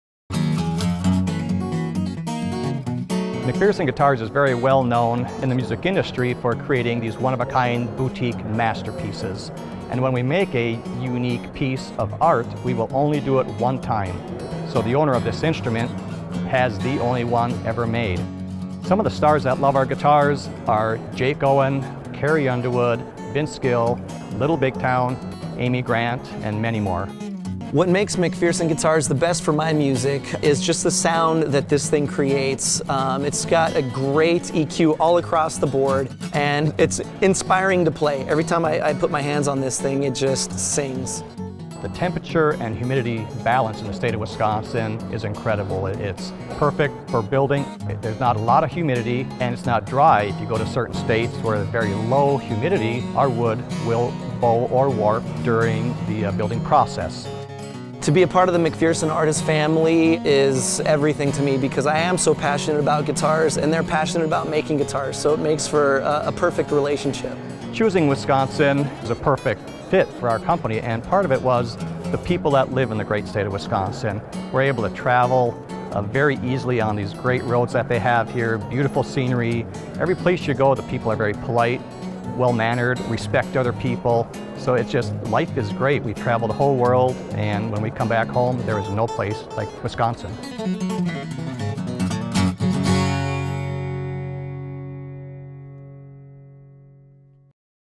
Wisconsin Stories: McPherson Guitars
Country stars like Carrie Underwood, Vince Gill and Little Big Town love Sparta’s own: McPherson Guitars! Each handmade guitar is a one-of-a-kind masterpiece. Check out their Wisconsin story